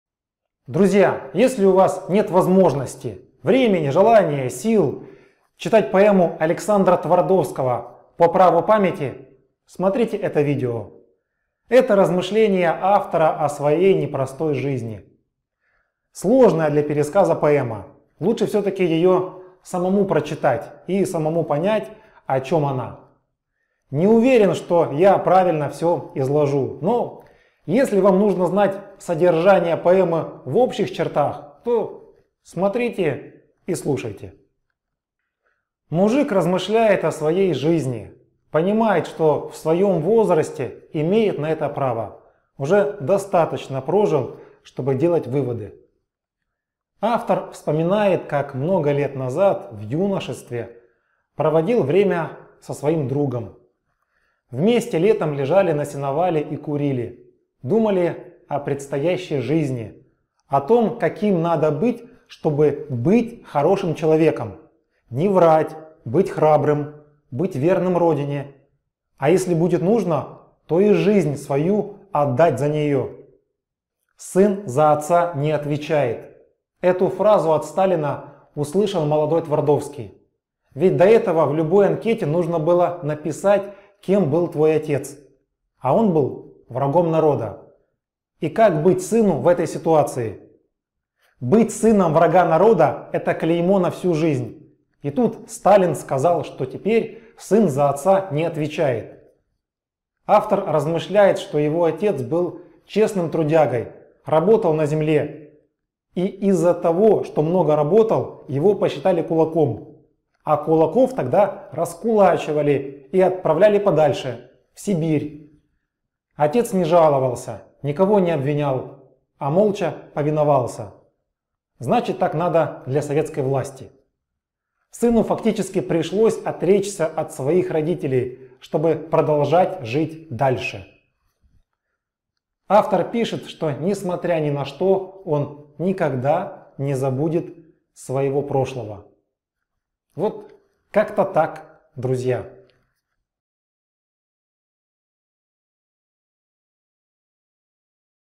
По праву памяти. Александр Твардовский
«По праву памяти» – за 2 минуты! Пересказ поэмы Александра Твардовкого.